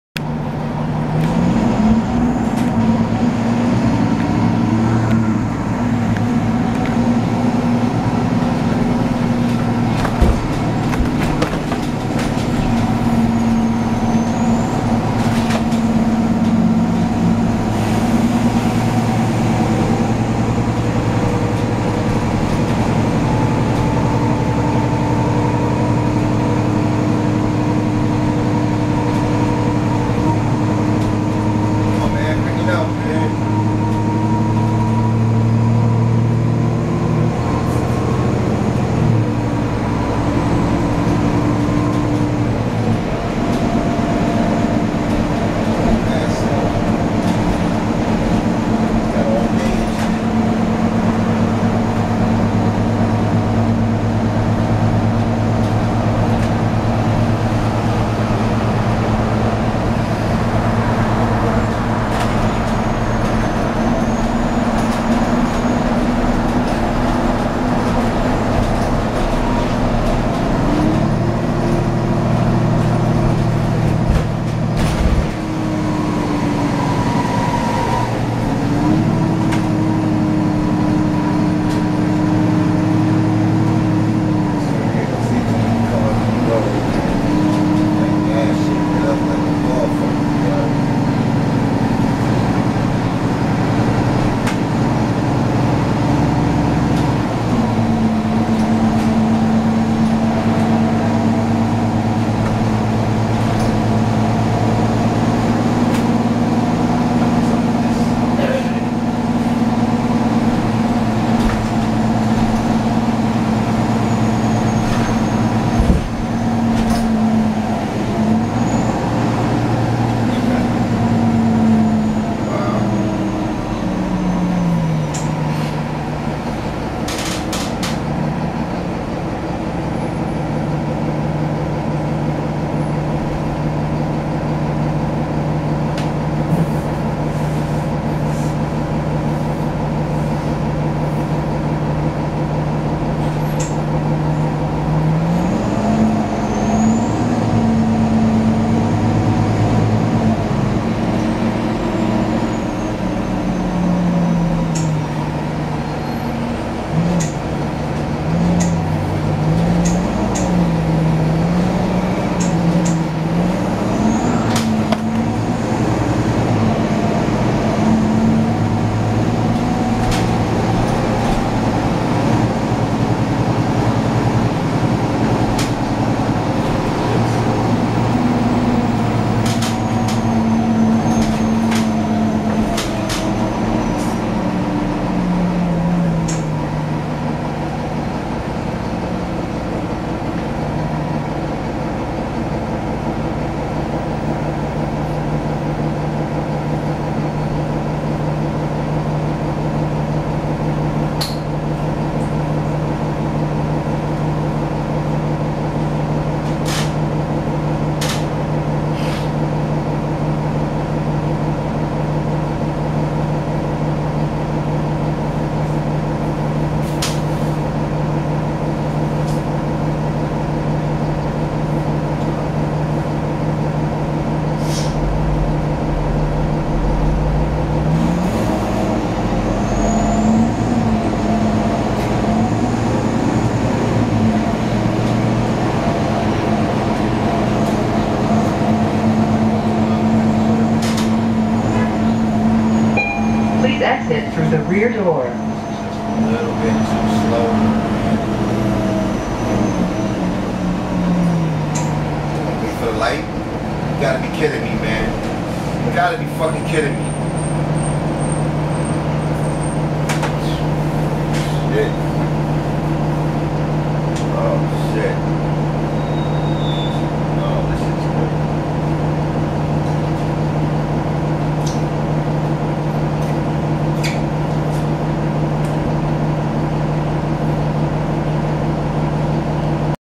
New York City Bus #5812 (The Ride)
On Route Bx15 to Harlem.

This is a 2012 NovaBus LFS-A (Articulated) with a Cummins ISL engine and  ZF EcoLife transmission.

NOTE: The comments made by a fellow passenger was the main reason I kept this video.